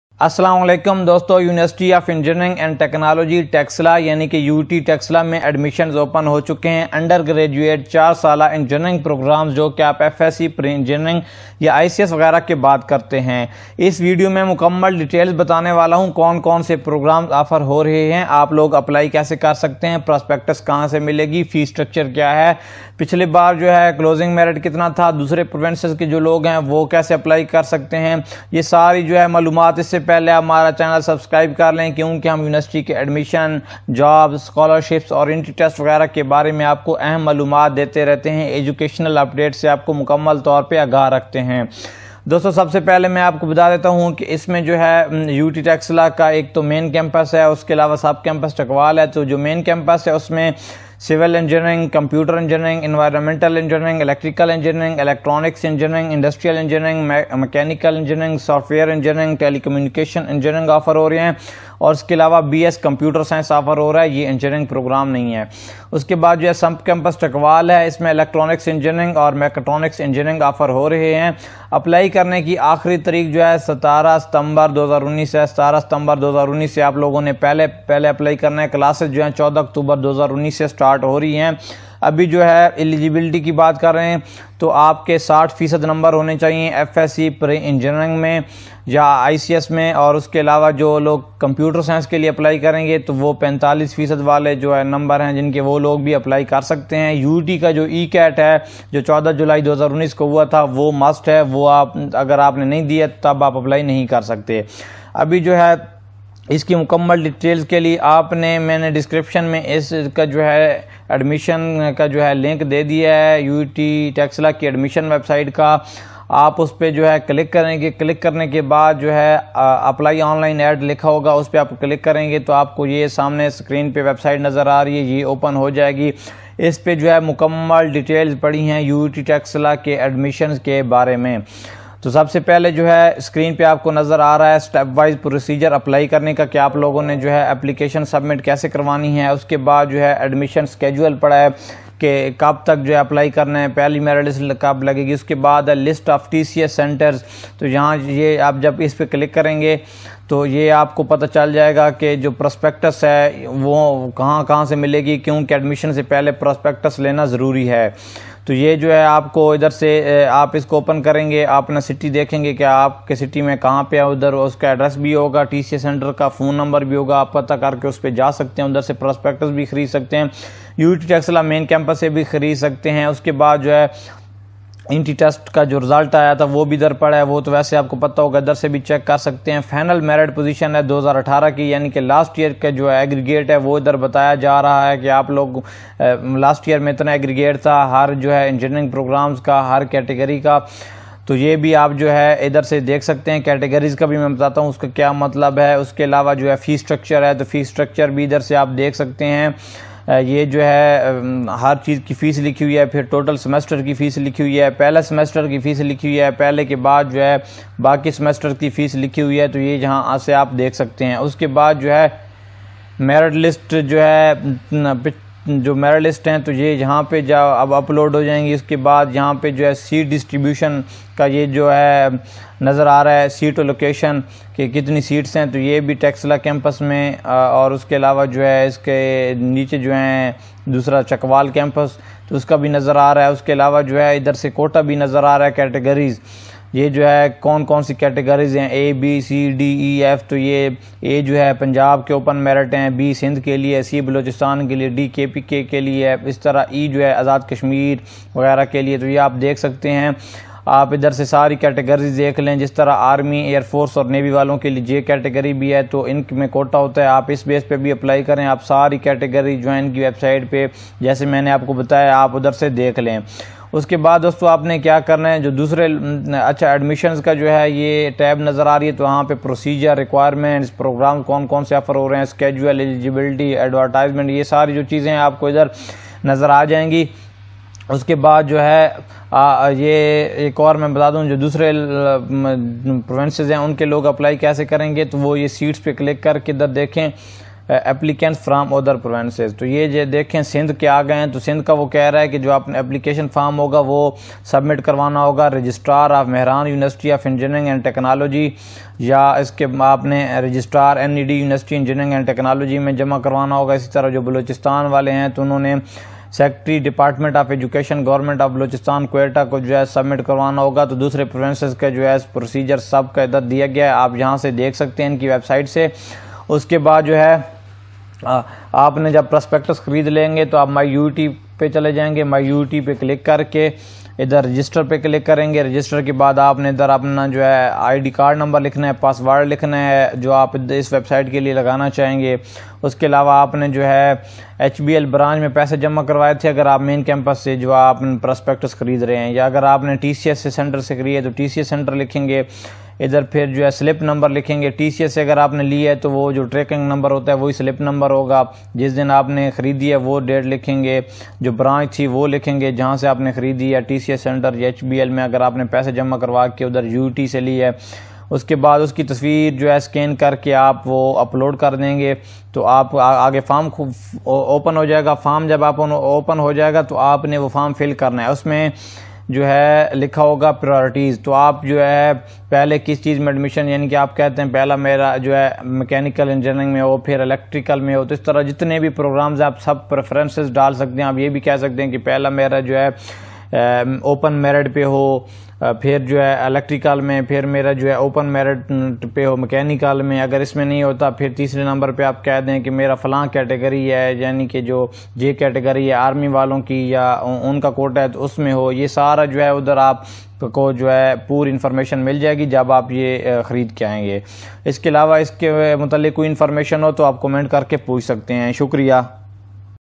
UET Taxila Undergraduate BSc Admissions 2019 | How to apply online in UET Taxila
In this video, I fully explained all information about University of Engineering and Technology UET  Taxila Undergraduate 4 Years BSc Engineering admissions including Program offered, How to apply, Eligibility Criteria, Critical Dates (Last Date to apply) merit lists date and prospectus availability etc.
UET Lahore offering BSc/BS admissions in all Engineering programs

University of Sargodha Admissions Fall 2019 | How to apply online in UOS BS MA MSc MS M.Phil PhD